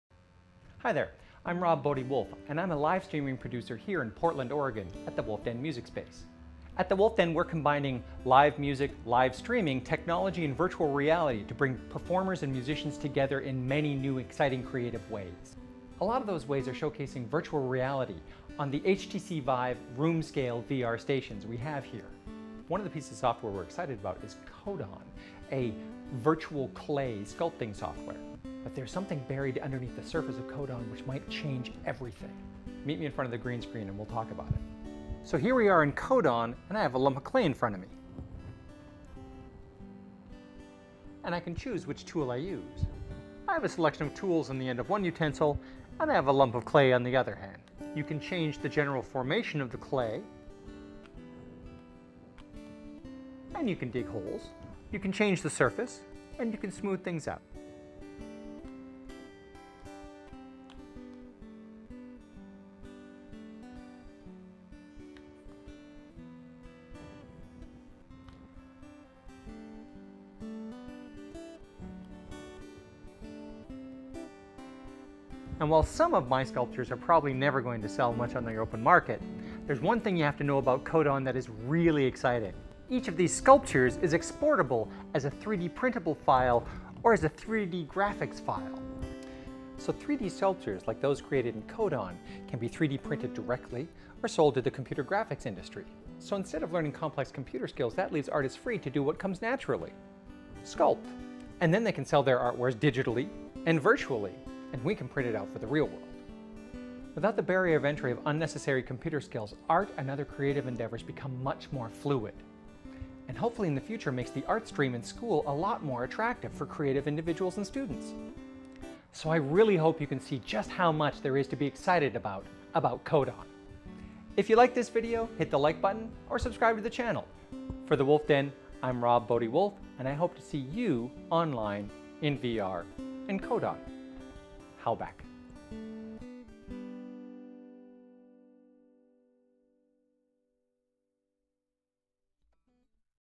Kodon in VR - Why we're So Excited
This video will showcase my HORRIBLE sculpting abilities again, but at least you'll get to see how amazing this virtual-clay sculpting software is!   And you can export in many useful file formats!